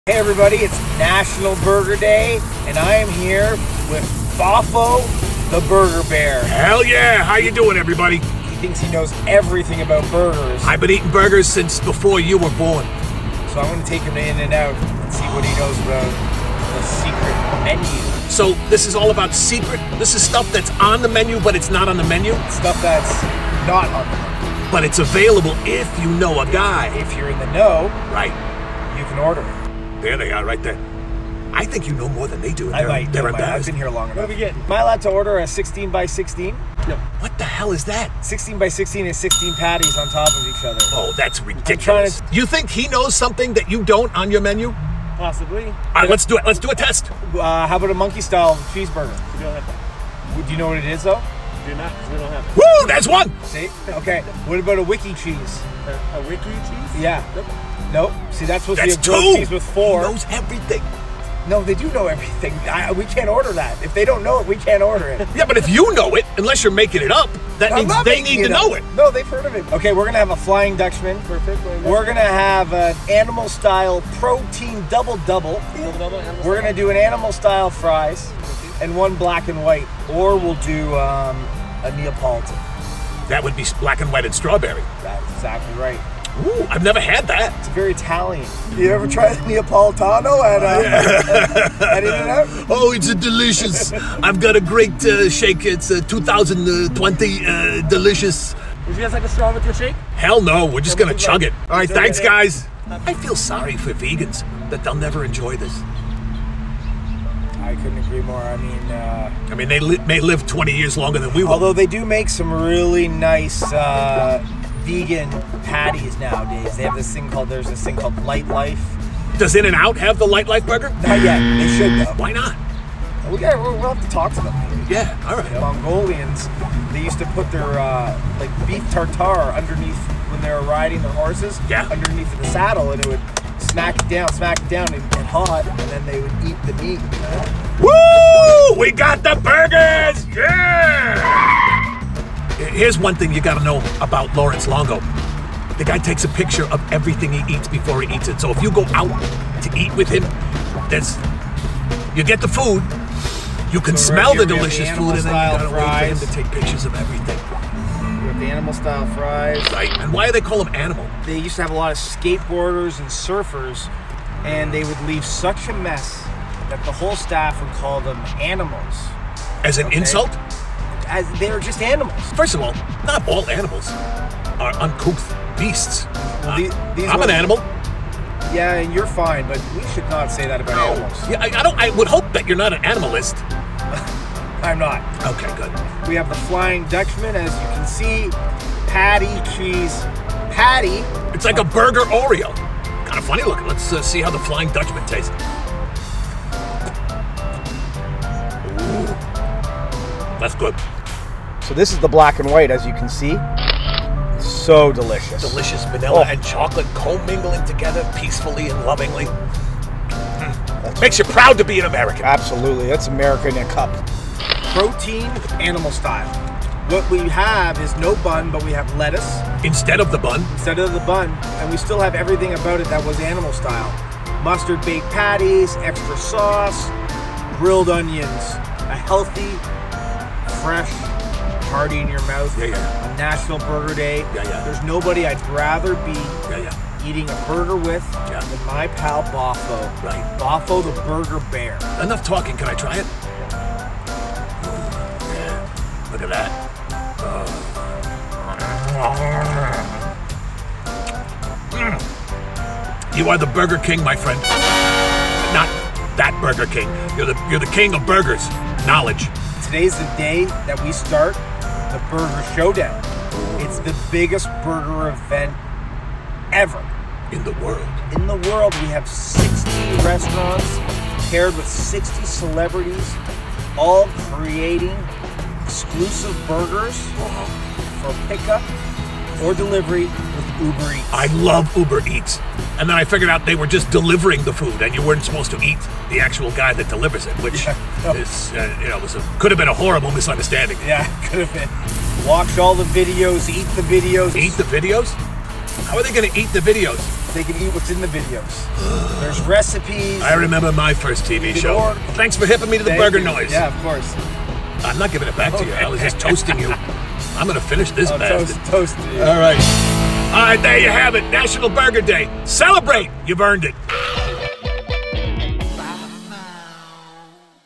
In N' Out SECRET MENU REVEALED!!! THEY DON'T WANT YOU TO SEE THIS!!!
In N' Out SECRET MENU REVEALED!!!
Lawrence Longo from Off the Menu Club teaches Boffo about all the secret items you can order at In n Out Burger. TOP SECRET! THEY DON'T WANT YOU TO SEE THIS!